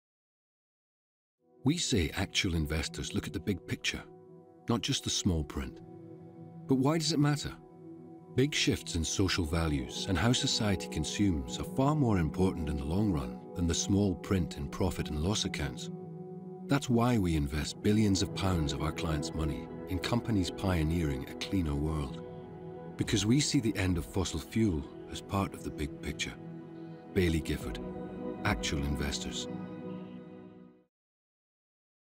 Actual Investors | The Big Picture | Baillie Gifford
Actual investing defines who we are and shapes every decision we make. It focuses us on the long term and it aligns the interests of investors, companies, and society in general.